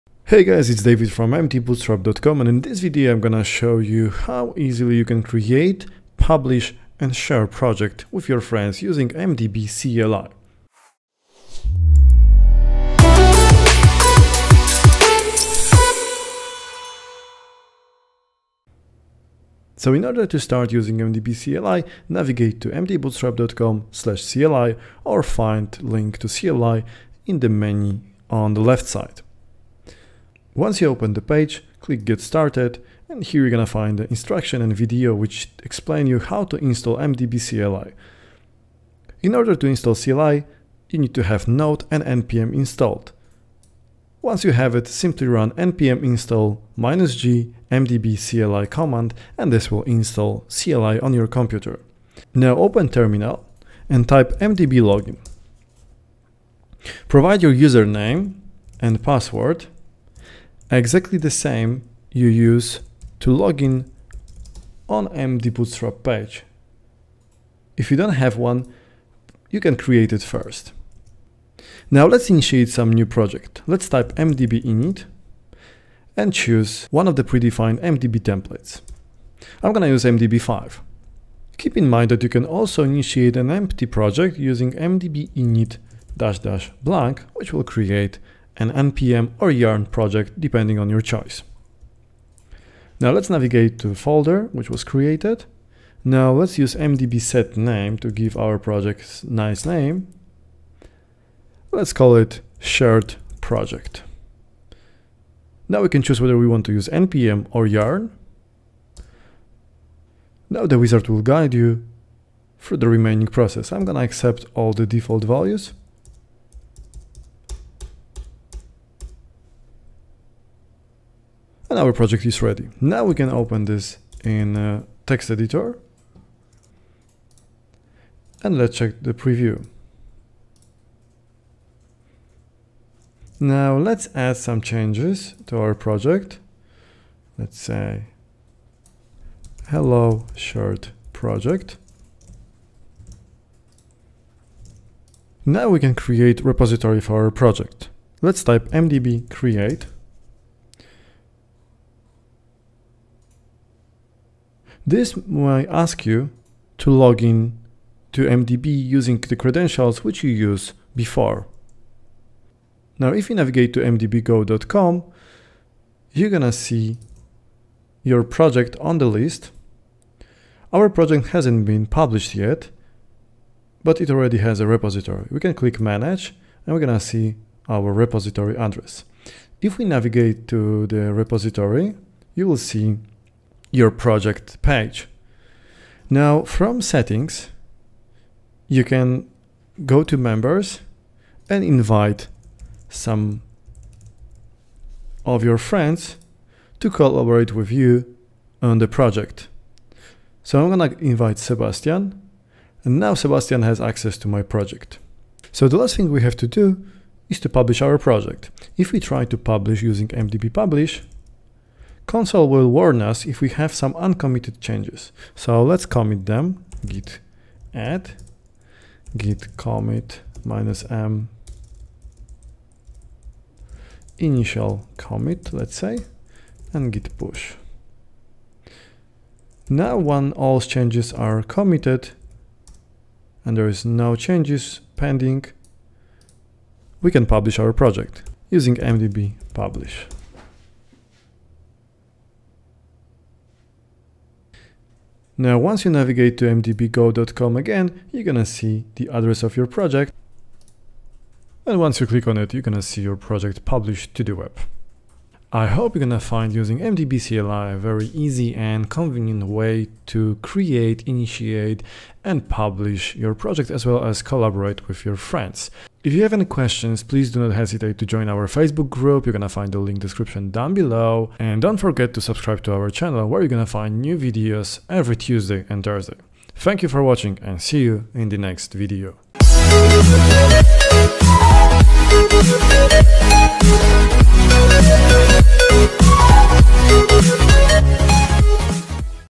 MDB Command Line Tutorial | Speed Up & Facilitate Project Creation
Create, deploy, and host your project with a single command. MDB CLI is a free tool that significantly speeds up and facilitates the creation of a new MDB project and its deployment.

➢ Creating MDB Project
➢ Deployment with a single command
➢Free hosting
➢ Easy management
➢Your own, custom domain

🎓 Learn responsive web design with the latest Bootstrap 5